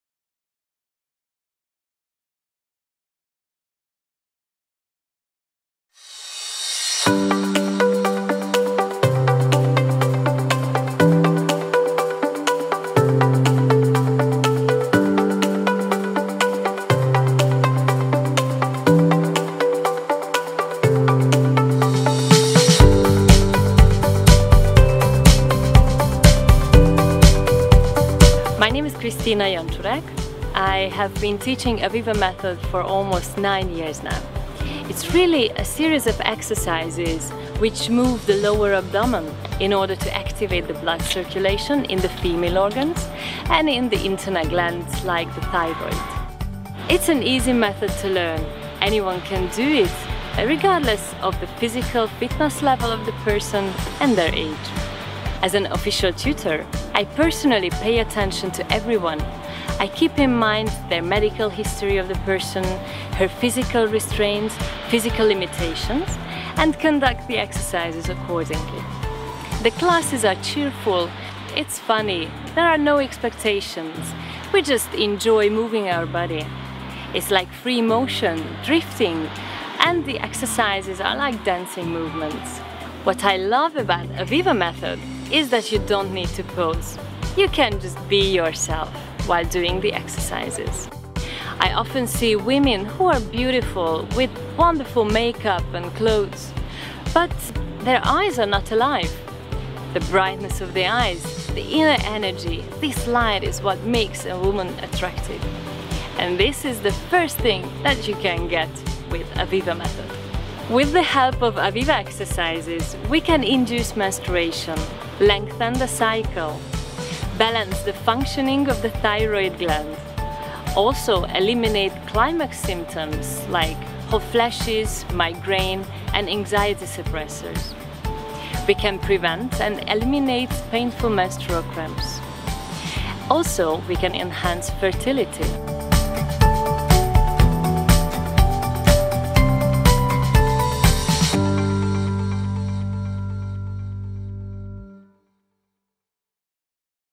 What is Aviva Method and why is it essential for every women?
Aviva gymnastics is an effective remedy for the elimination of gynecological and urological diseases, for treating infertility and for solving hormonal problems such as malfunctions of the thyroid gland, adrenal gland and pituitary gland, discomfort or excess weight. This type of exercise offers a natural solution for both men and women to initiate the self-healing processes of their bodies and take the improvement of their health into their own hands. The gymnastics itself is just part of the Aviva Method complex toolkit, which includes teaching you how to breathe, exercise, and eat properly. So, an officially trained Aviva method instructor will provide practitioners with useful and personalized lifestyle tips alongside gymnastics exercises to help you heal and achieve your goal as quickly and efficiently as possible.